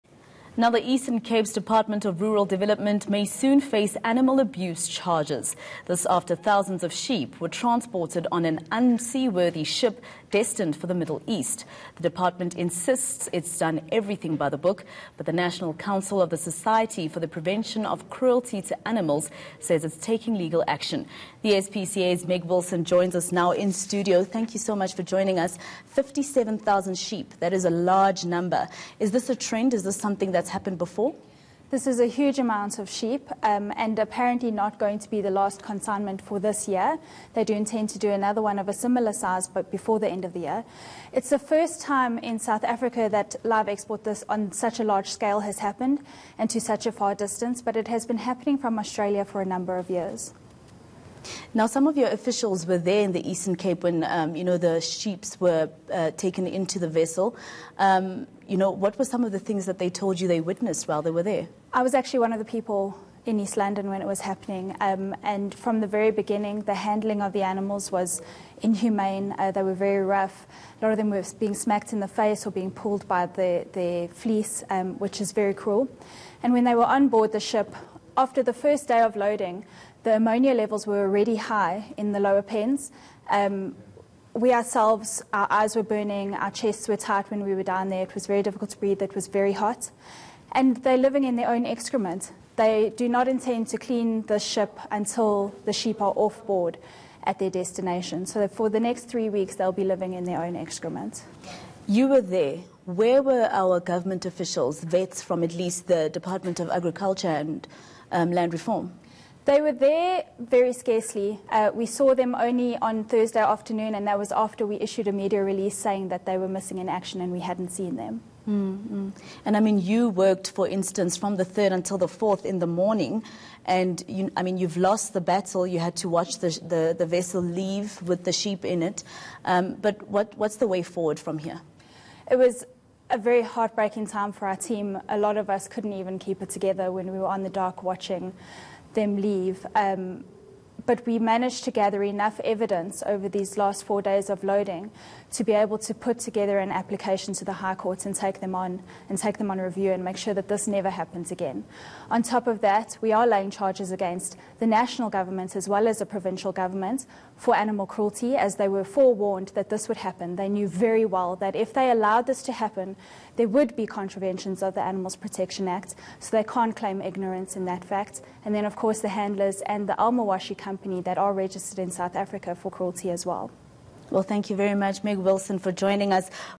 NSPCA to sue Eastern Cape department
The Eastern Cape's Department of Rural Development may soon face animal abuse charges. This after thousands of sheep were transported on an unseaworthy ship destined for the Middle East. Courtesy